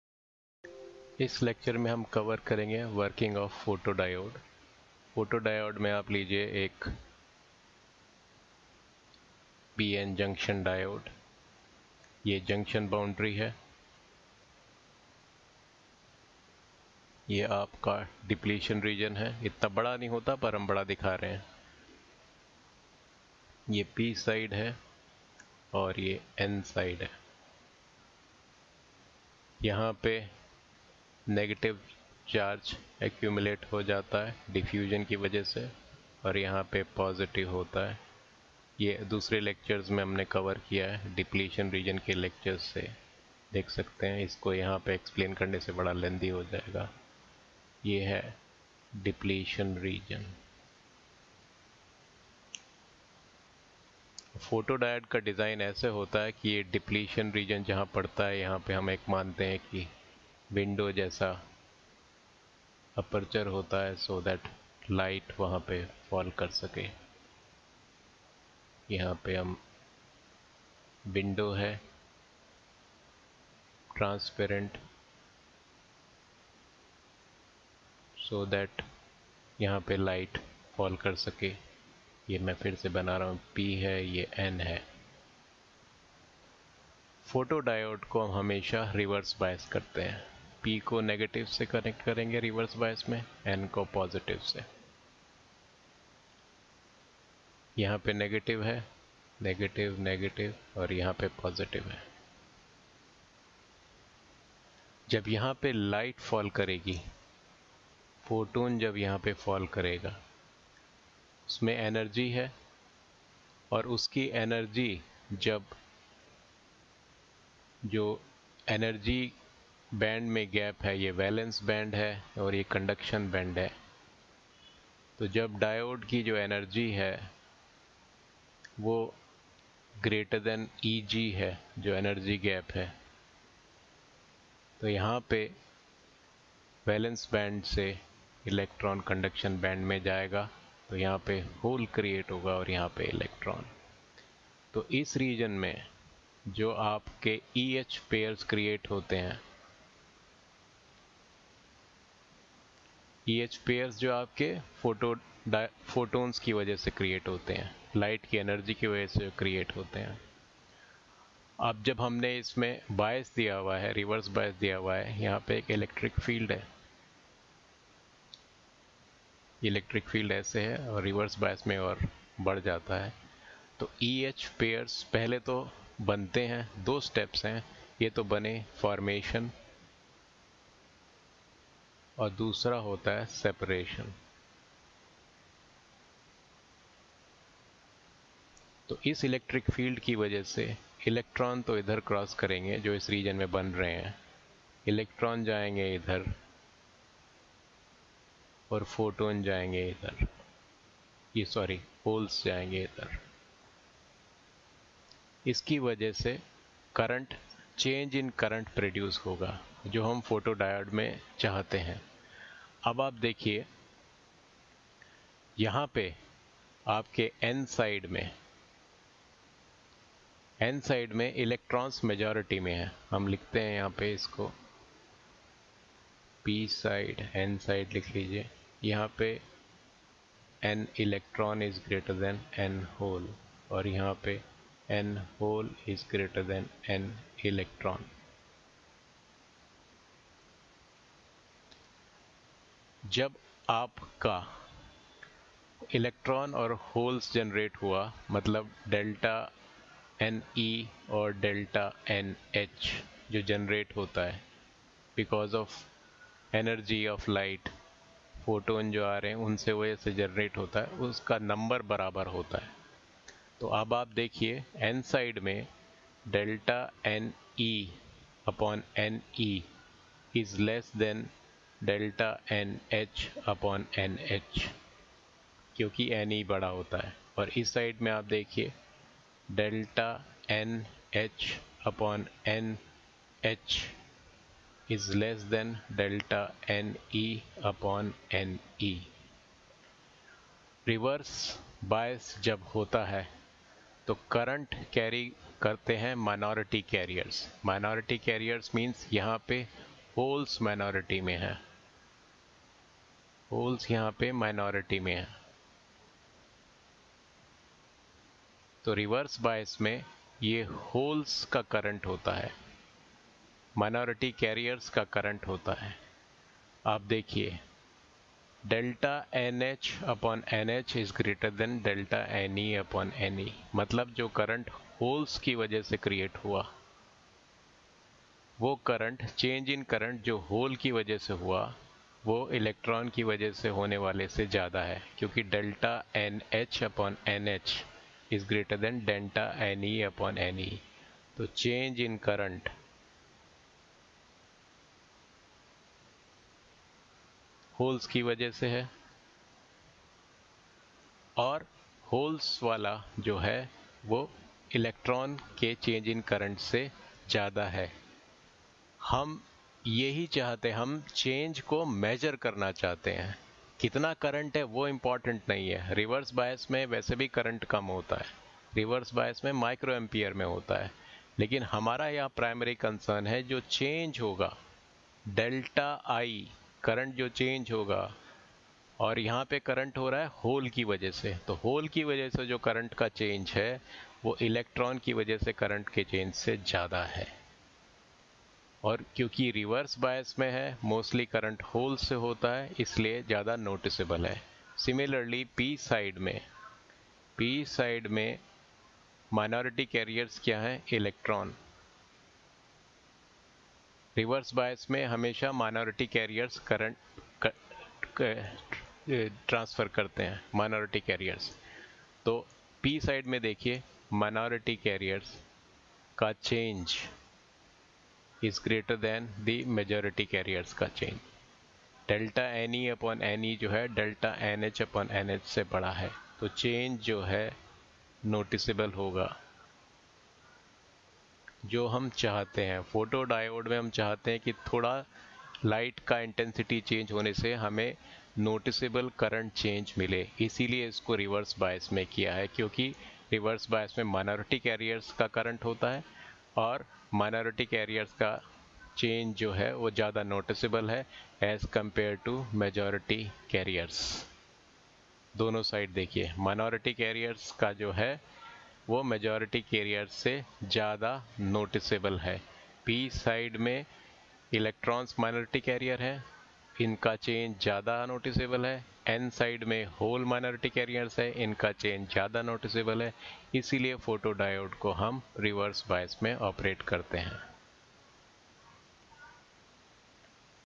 why photo diode is operated in reverse bias class 12 physics in Hindi
By Y. Kumar, ex IES( Mechanical AIR 1), Alumnus UoR(IIT Roorkee)